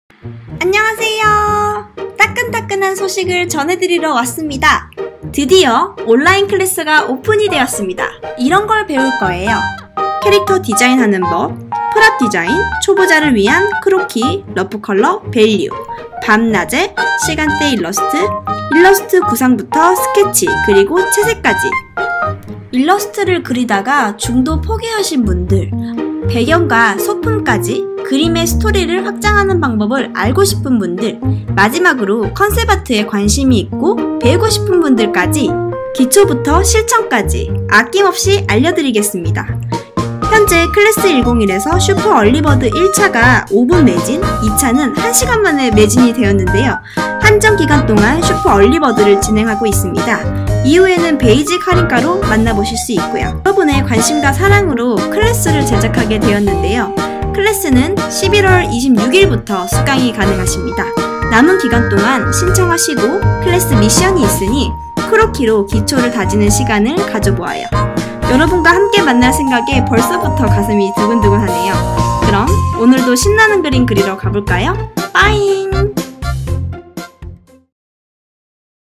🎨 효나효나 클래스101 얼리버드 오픈 ! !👩‍🎨 그램쟁이들 모여라!
클래스101에서 [ 컨셉아트 클래스] 오픈 했어요!💖
애니메이션 컨셉아트의 캐릭터, 소품, 배경 , 일러스트 등
양과 질로 정성 담아 열심히 준비했습니다.

1차 얼리버드가 5분 / 2차는 1시간만에 매진 되었습니다 ㅠㅠ
남은 한정기간 판매중이오니 서둘러주세요!

11월26일부터 수강 가능합니다!
  ✨ 온라인 클래스 101 수강신청!